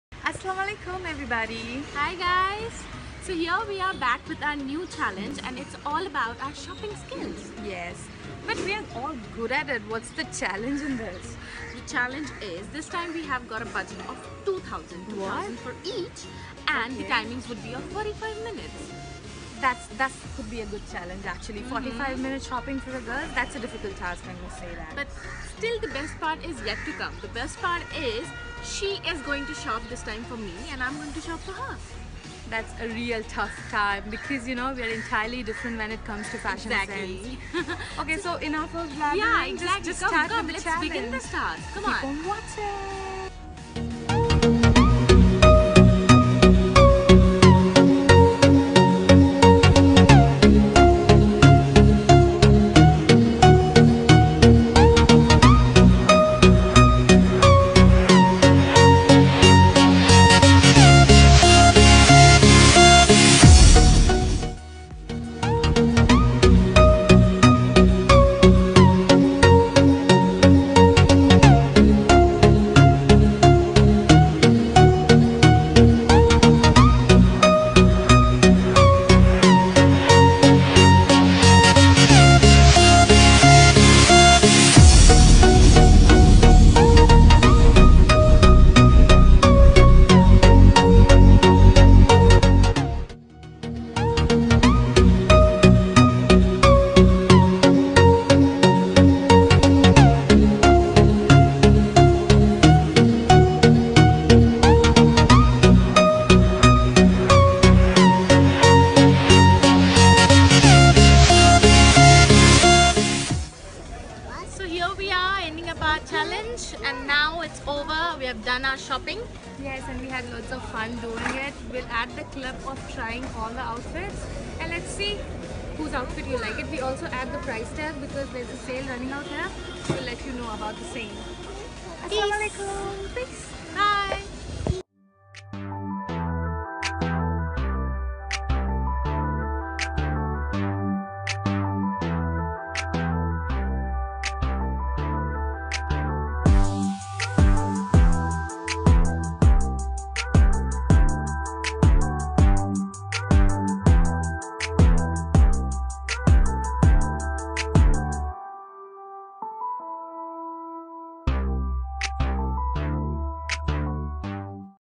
Sister tag
Hello guys we are back with another video. It's a fun challenge where we sisters are gonna shop for each other with a budget of only ₹2000 with a time limit of 45 minutes.
We tried the items please let us know the item you liked the most.

We have listed all the products with there original price and the discounted price as well.